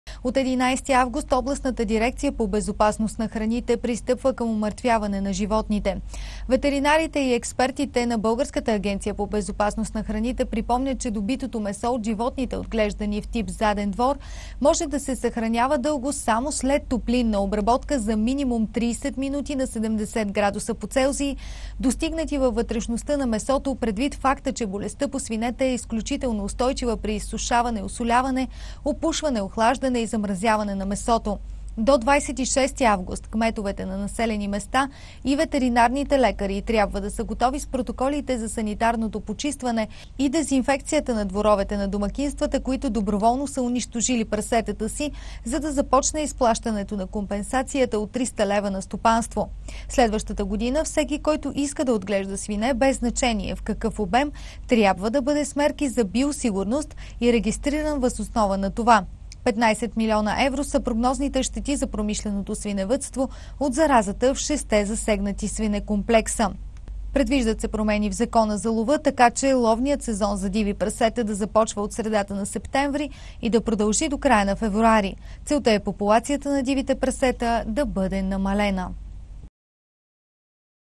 Изтече срокът за доброволното умъртвяване на прасетата
В неделя изтече срокът, в който стопаните от Южна България трябваше доброволно да изколят прасетата в личните си стопанства като мярка за справяне с разпространението на болестта африканска чума по свинете. От 11 август Областната дирекция по безопасност на храните пристъпва към умъртвяване на животните от. Ветеринарите и експертите на БАБХ припомнят, че добитото месо от животните, отглеждани в тип „заден двор” може да се съхранява дълго само след топлинна обработка за минимум 30 минути на 70°С, достигнати във вътрешността на месото, предвид факта, че болестта по свинете е изключително устойчива при изсушаване, осоляване, опушване, охлаждане и замразяване на месото. До 26 август кметовете на населени места и ветеринарните лекари трябва да са готови с протоколите за санитарното почистване и дезинфекцията на дворовете на домакинствата, които доброволно са унищожили прасетата си, за да започне изплащането на компенсацията от 300 лева на стопанство. Следващата година всеки, който иска да отглежда свине, без значение в какъв обем, трябва да бъде с мерки за биосигурност и регистриран въз основа на това. 15 милиона евро са прогнозните щети за промишленото свиневъдство от заразата в шестте засегнати свинекомплекса. Предвиждат се промени в Закона за лова, така че ловният сезон за диви прасета да започва от средата на септември и да продължи до края на февруари. Целта е популацията на дивите прасета да бъде намалена